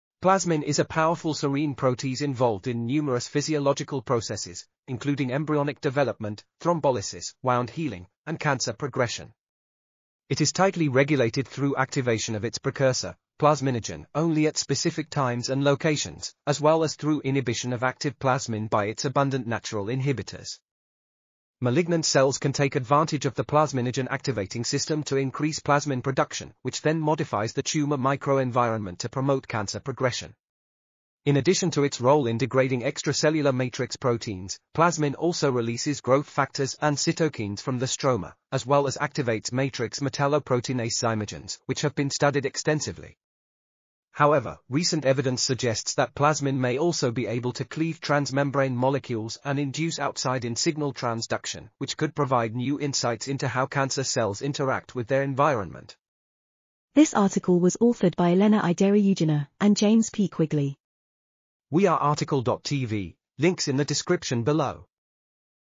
Cell Surface Remodeling by Plasmin: A New Function for an Old Enzyme | RTCL.TV
### Keywords ###

### Article Attribution ###
Title: Cell Surface Remodeling by Plasmin: A New Function for an Old Enzyme
Authors: Elena I. Deryugina ,and James P. Quigley
Publisher: Hindawi Limited
DOI: 10.1155/2012/564259

### Video Timestamps ###
0:00:00 - Summary
0:00:56 - Title
0:01:01 - Outro
0:01:05 - End